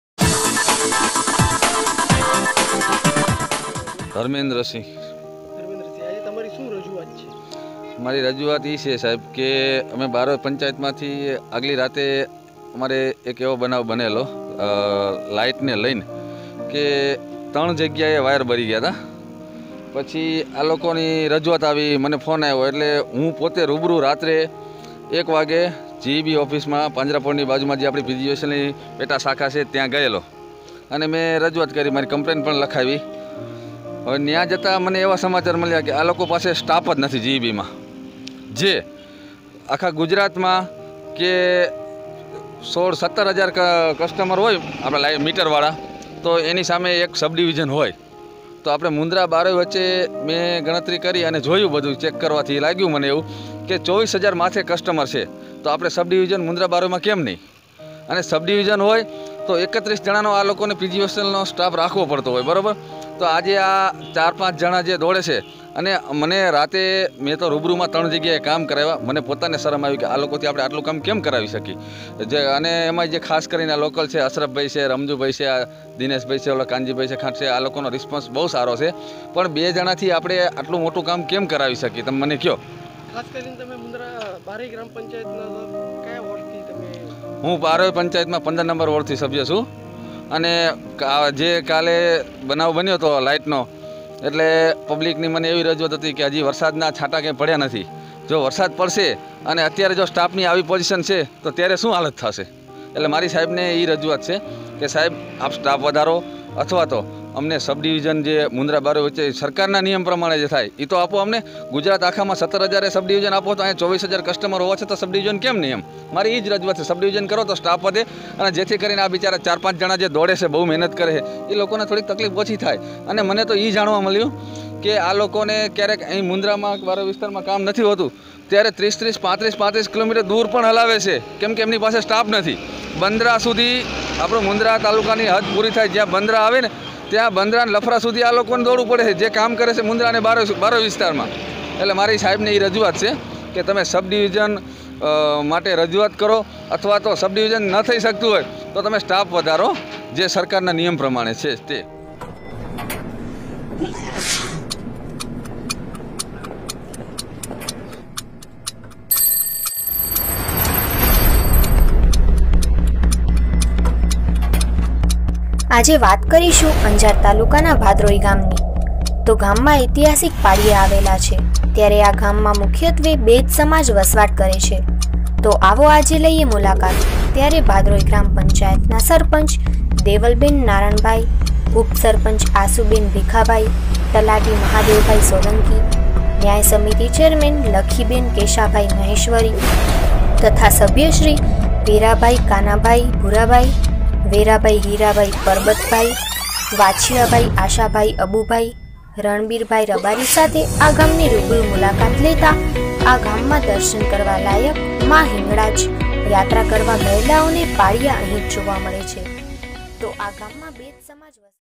◆ બારોઇ ગ્રામ પંચાયતના સભ્ય ધર્મેન્દ્રસિંહ દ્વારા G.E.B. ને અનુલક્ષીને આવેદન આપવામાં આવ્યું.
◆ ક્ચ્છ કાનૂન એન્ડ ક્રાઈમ ન્યૂઝ